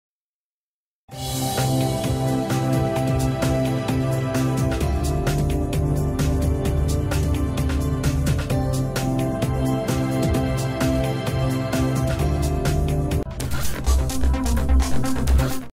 Fuego arrasó con un vehículo en horas de la madrugada en Iquique
Personal de bomberos se trasladó al sitio de la emergencia y lograron controlar la situación.